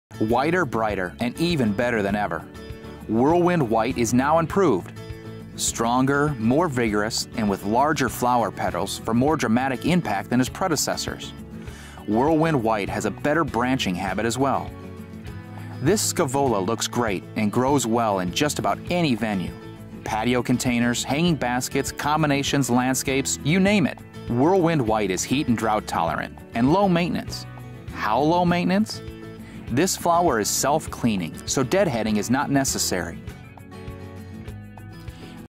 Proven Winners® Gardener Channel: Proven Winners® Whirlwind® White Scaevola
Don't be fooled by their delicate, lacy flowers and gracefully mounding/spreading habit. Fan flowers are native to the Bush country of Western Australia. Constantly in flower, these combo stars also boast attractive foliage and a well-branched habit. Whirlwind White continues to set the standard for season-long performance and immunity to temperature extremes and tough conditions.